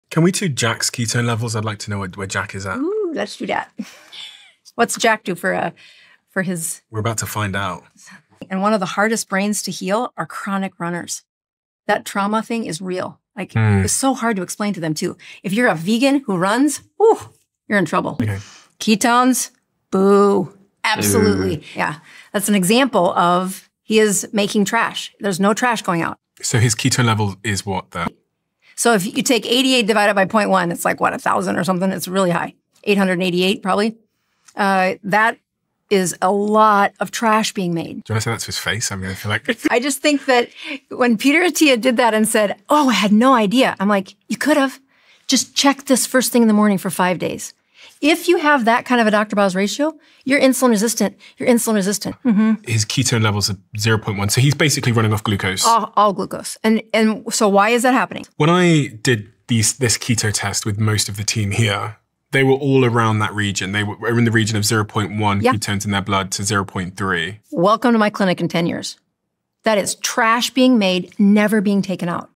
STOP MAKING TRASH! The shocking ketone number that screams INSULIN RESISTANCE. 🤯
What happens when a seemingly healthy individual's blood work tells a completely different story? This clip exposes a startling analysis where a measured blood ketone level of 0.1 is interpreted as an alarming sign of metabolic dysfunction—a huge amount of cellular "trash" being generated with no clear pathway for removal. The discussion brings the implications of the often-discussed "doctor-boss ratio" to the surface, suggesting that this simple metric could be an immediate red flag for insulin resistance. Furthermore, the segment touches upon the difficulty in healing the brains of chronic runners, particularly those maintaining a vegan diet, questioning established health assumptions. An expert perspective reveals that consistently low blood ketones might indicate the body is running entirely on glucose, a physiological state that warrants immediate attention for long-term well-being.